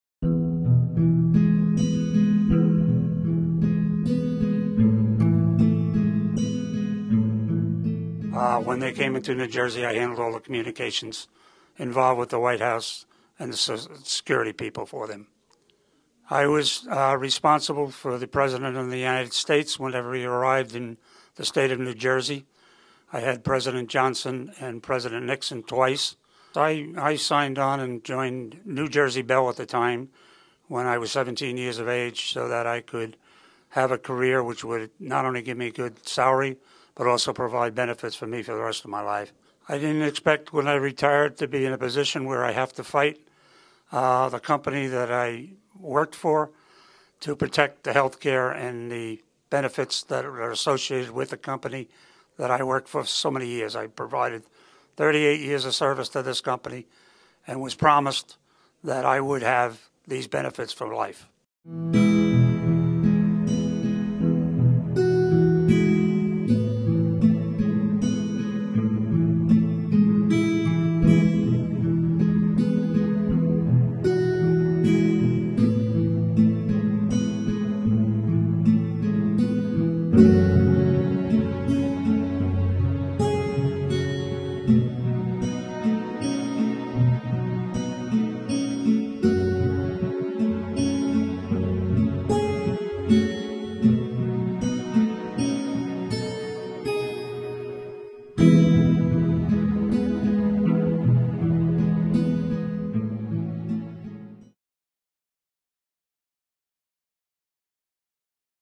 Chapter 3: Signed On At Seventeen
A new video documentary, Retiree Chronicles: Happy Memories, Uncertain Futures, features retirees from the American telecommunications industry reminiscing about careers in the Bell System, and the economic uncertainty and betrayal they suffered in the face of broken commitments from their former employer.